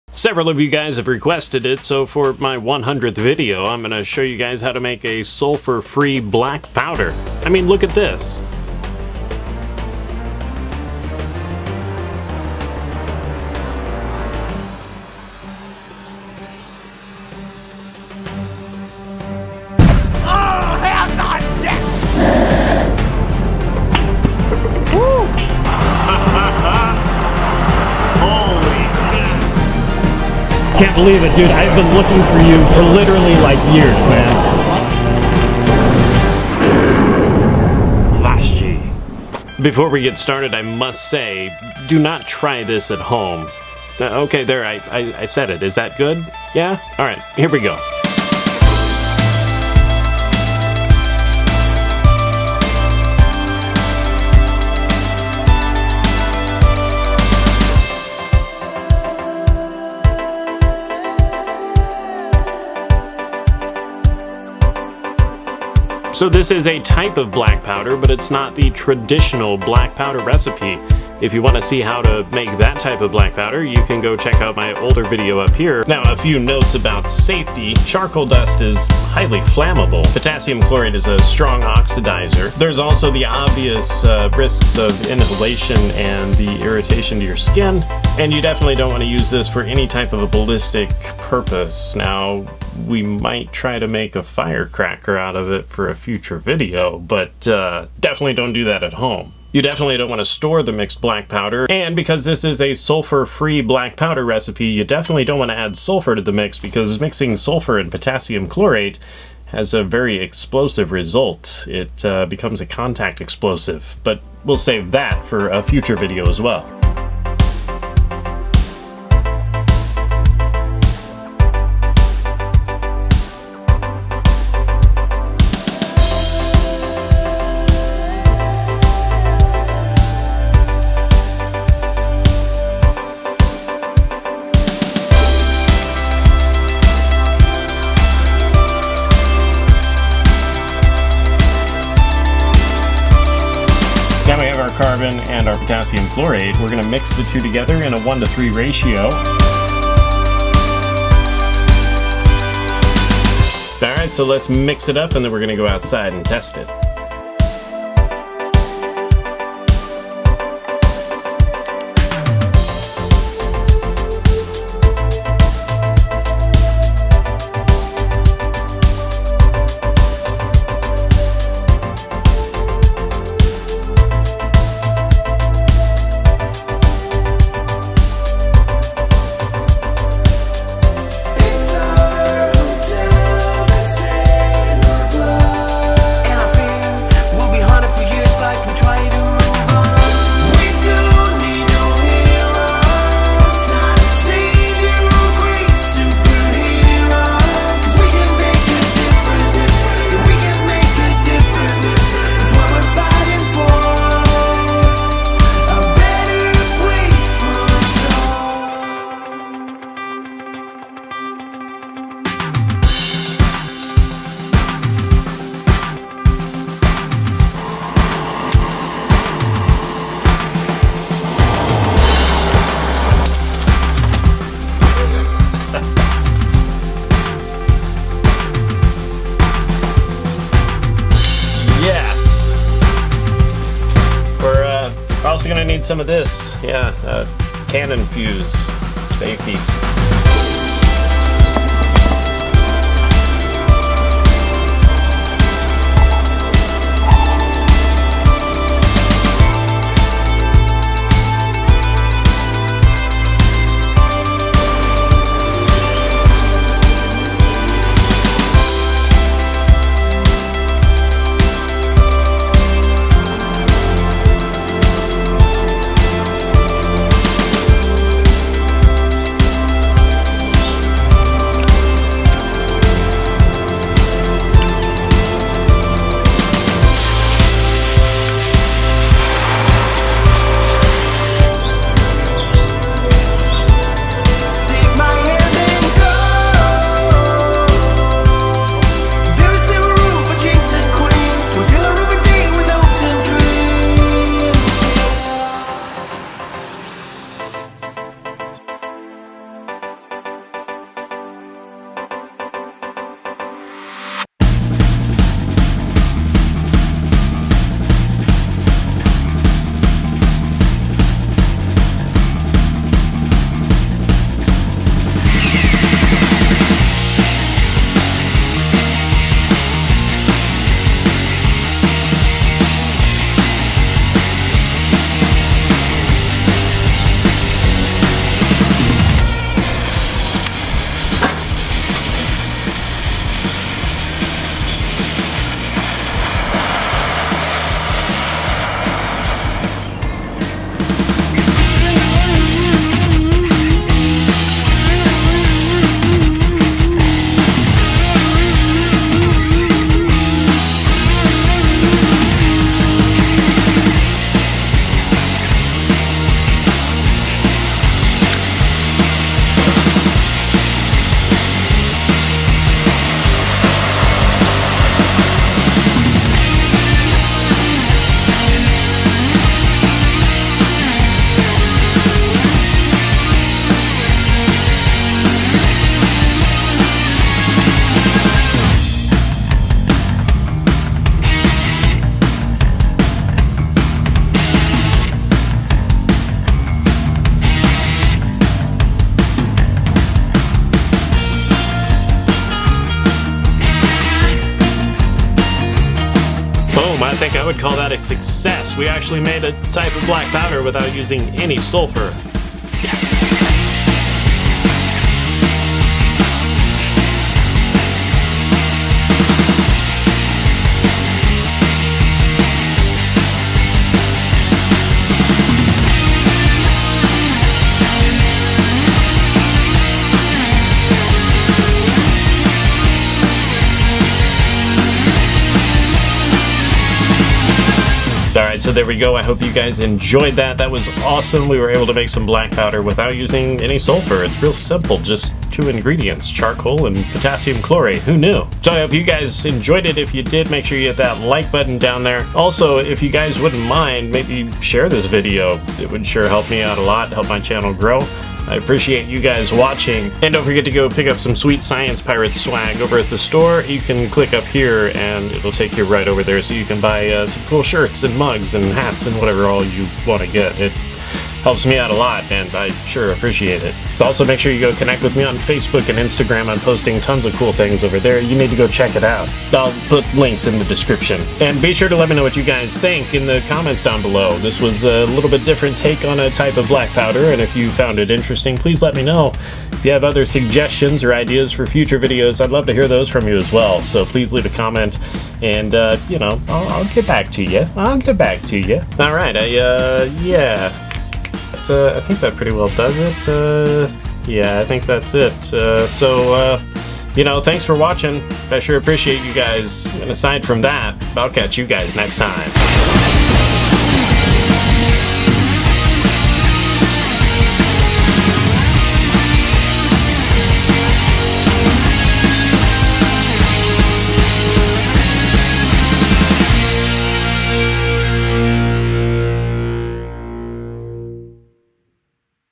Making Black Powder (Gunpowder) Without Sulfur! (Carbon & Potassium Chlorate Powder)
Exploring a simpler method for making a Black Powder (like gunpowder) using only two ingredients!  Mixing carbon and potassium chlorate provides a great black powder that burns fairly rapidly and surprisingly produces a lot of smoke!  This is more of a "black powder" than a gunpowder, as this isn't suitable for ballistic use.  This is the most simple black powder recipe I've used and the result is very satisfying.  Please DO NOT try to make your own black powder at home.  Remember, I am a trained professional!

I couldn’t link to the original “Making Black Powder” video within this video, so here’s the